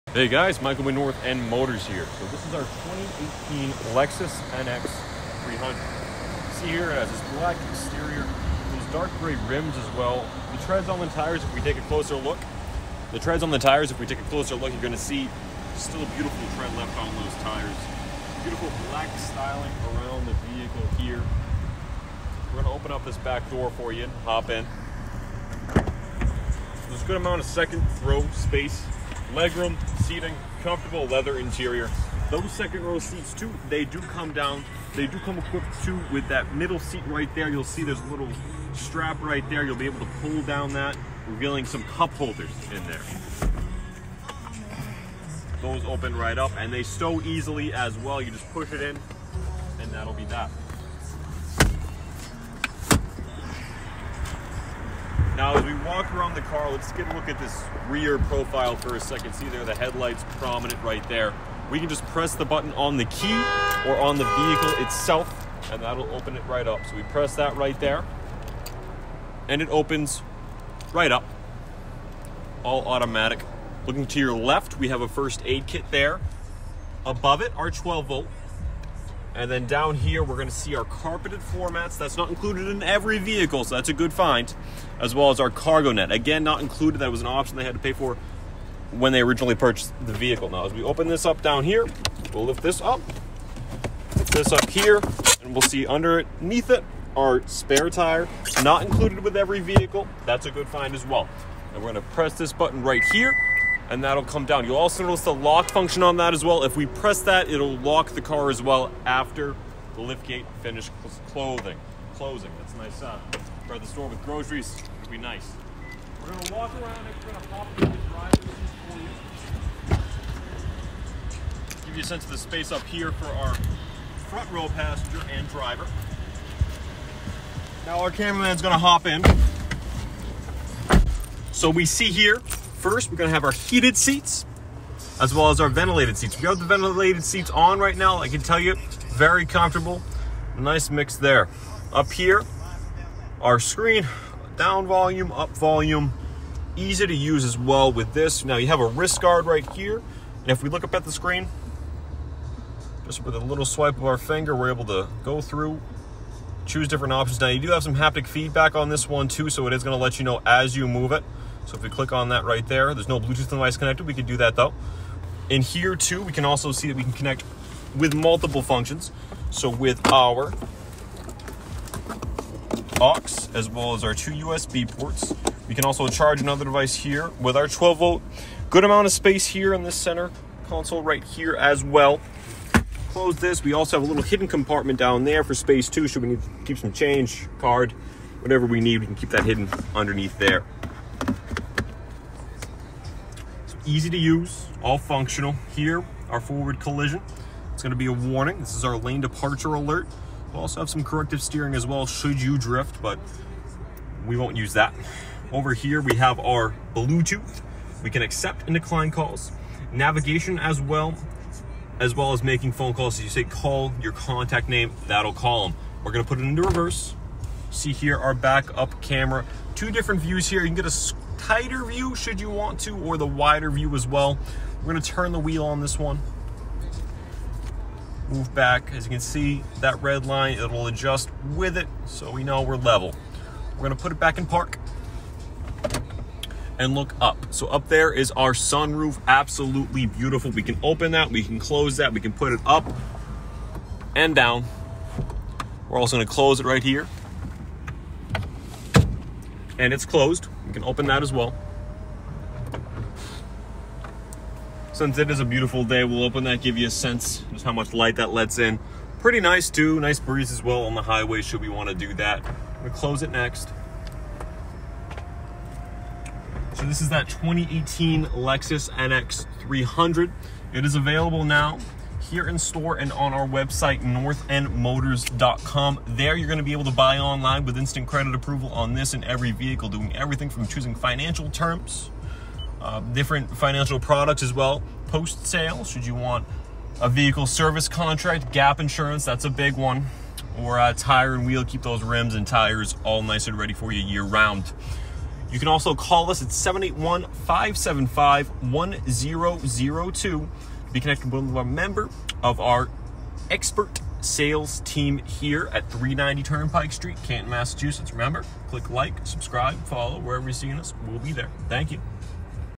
2018 Lexus NX 300
Take a dive into our available 2018 Lexus NX 300!

With heated/ventilated front seats, easy to use luxury infotainment system and more!